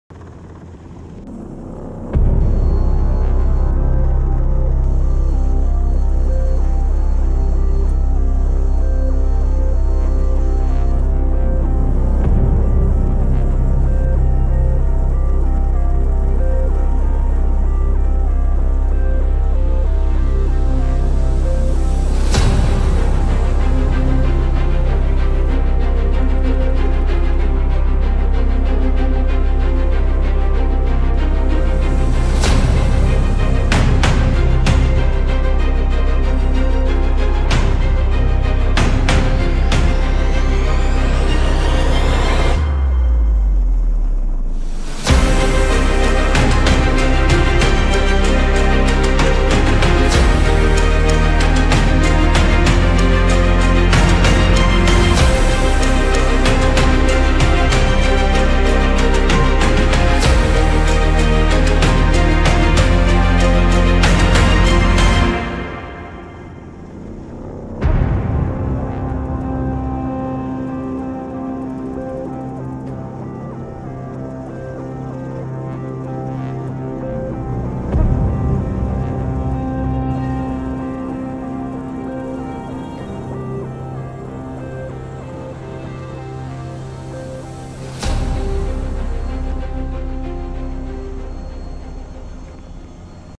Stretcher view during a hoisting by Belgian Air Force NH90 SAR.
Enjoy the view when you are picked up by the 40SQN NH90 SAR.